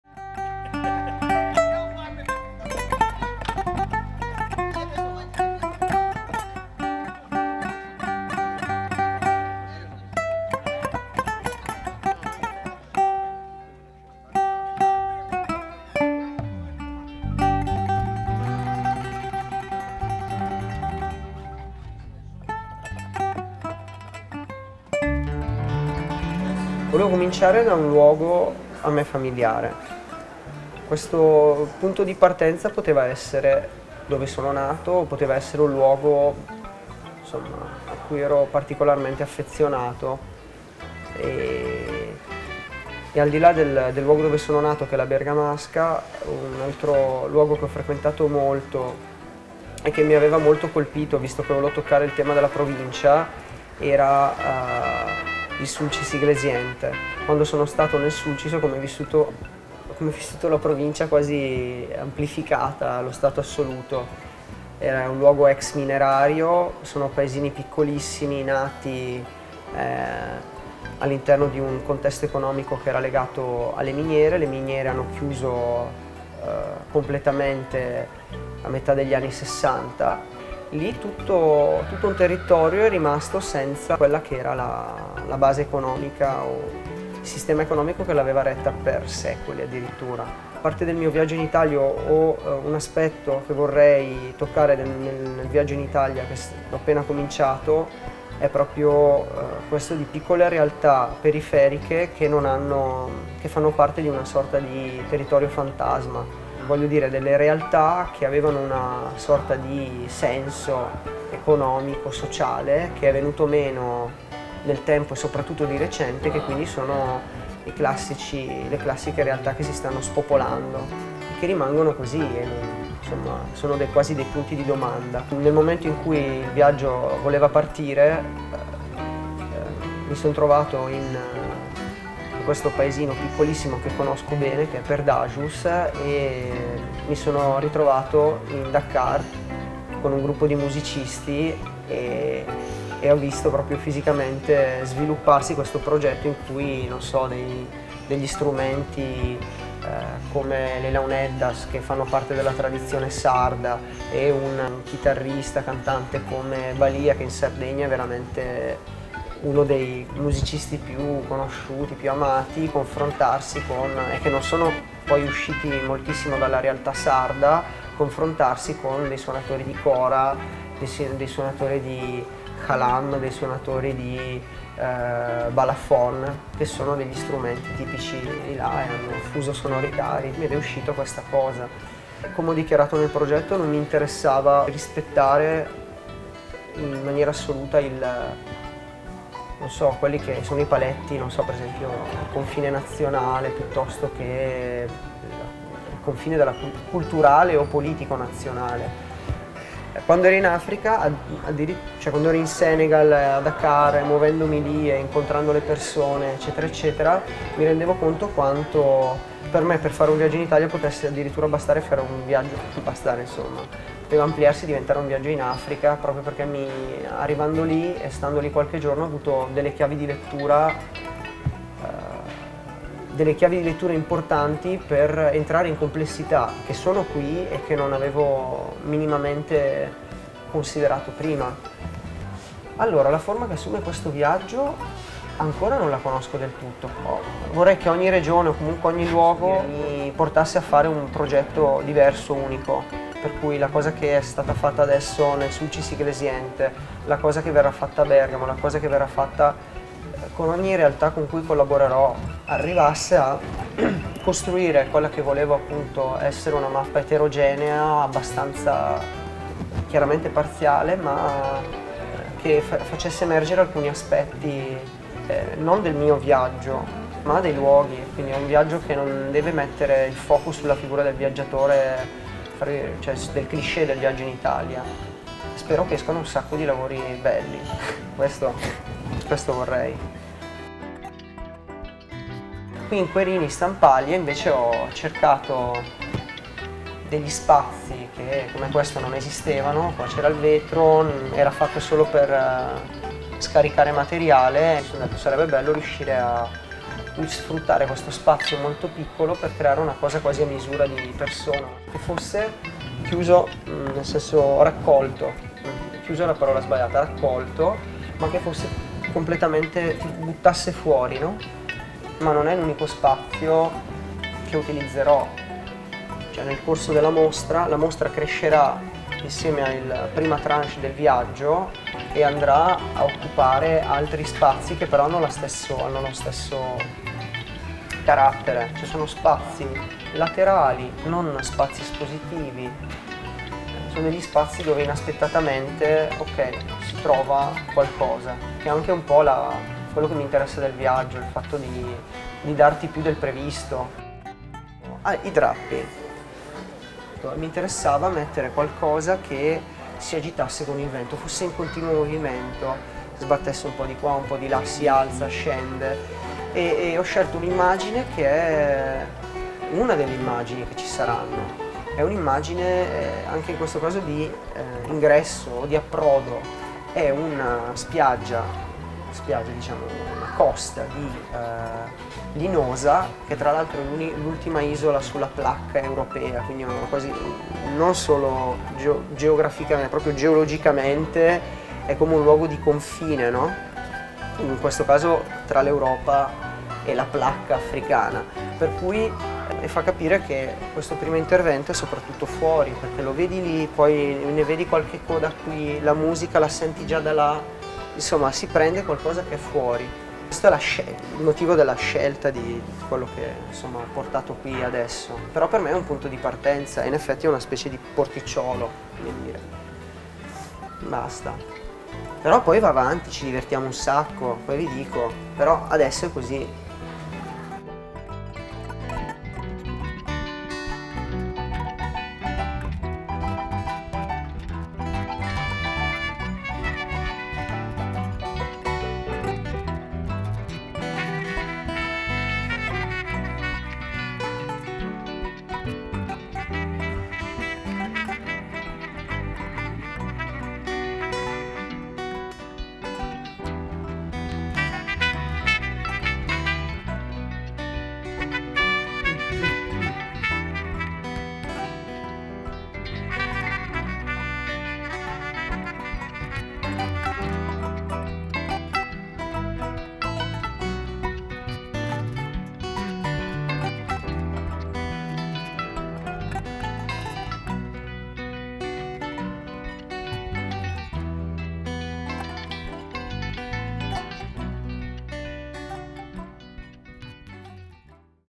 Matteo Rubbi: Viaggio in Italia, Furla Art Award 2011
Matteo Rubbi introduces the exhibition Viaggio in Italia, his project that won the 8th 2011 Furla Prize.
In a small area of the Palazzo Querini Stampalia the artist creates a place in which to take a break and discover news of the trip in progress.